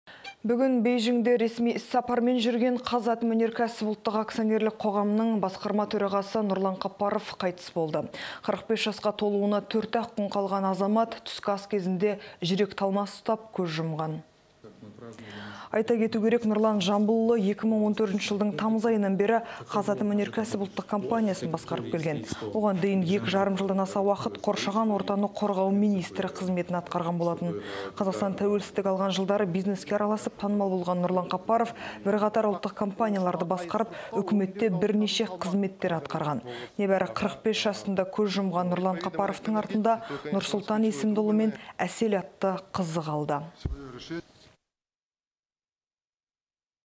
Қаппаров қайтыс болды
Бүгін Бейжіңде ресми іс сапармен жүрген "Қазатомөнеркәсіп" ұлттық акционерлік қоғамының Басқарма төрағасы Нұрлан Қаппаров қайтыс болды. 45 жасқа толуына төрт-ақ күн қалған азамат түскі ас кезінде жүрек талмасы ұстап, көз жұмған.